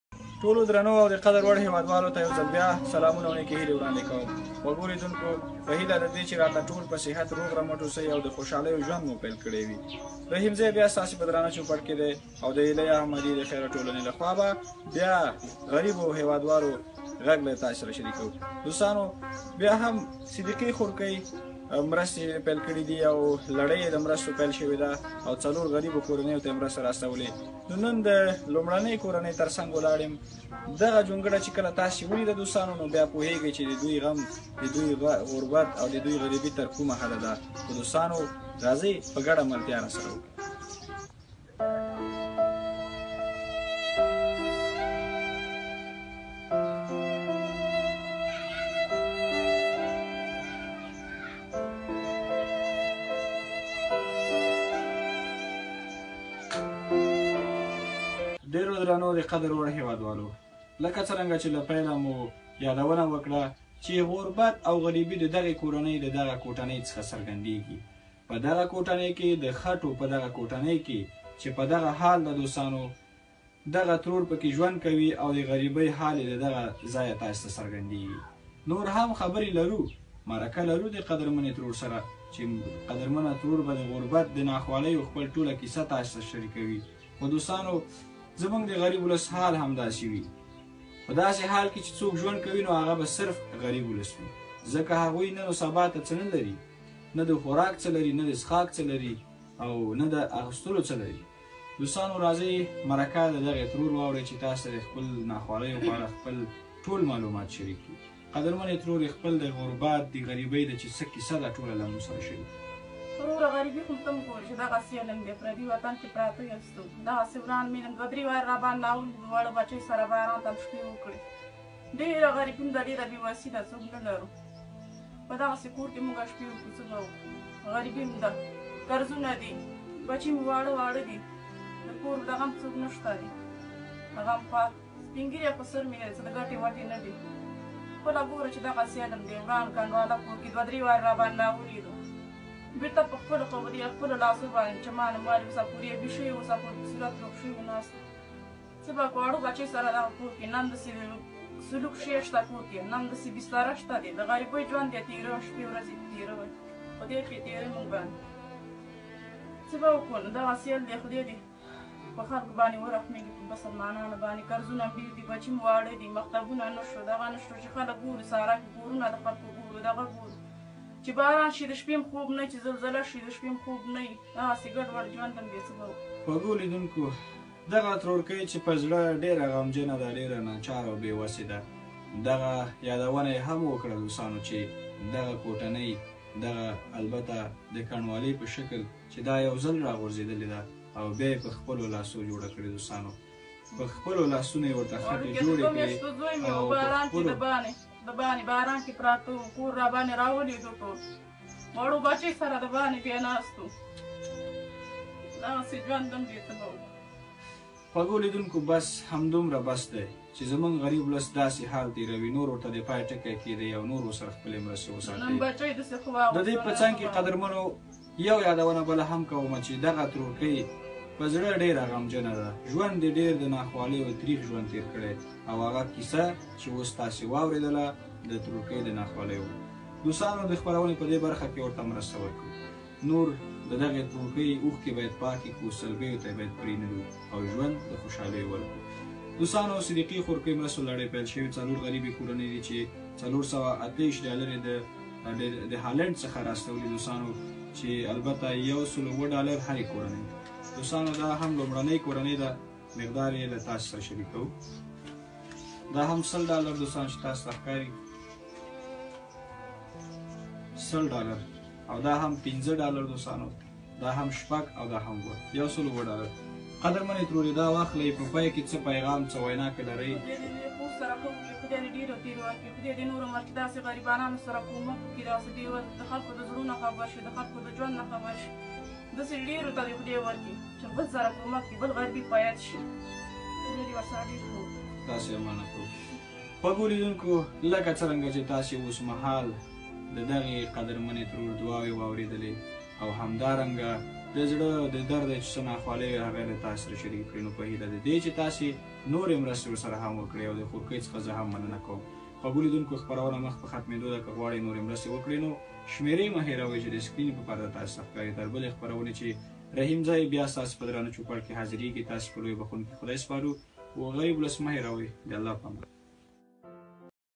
Afghanistan poor people's
The Foundation of Helai Ahmadai is a new grant-giving trust that provides funding for women, poor families and children in Afghanistan 🇦🇫